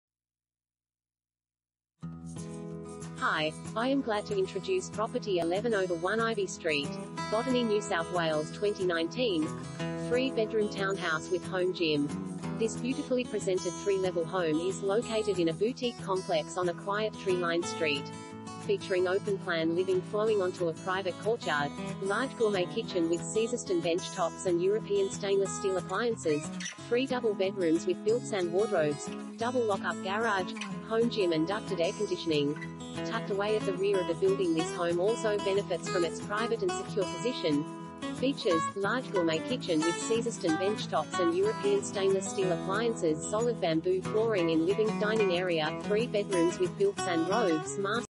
11/1 Ivy Street, Botany NSW 2019 | AuPropertyReport.Com
11/1 Ivy Street, Botany NSW 2019
Property Type: townhouse
Bedrooms: 3
Bathrooms: 3
Car Space: 2
3 BEDROOM TOWNHOUSE WITH HOME GYM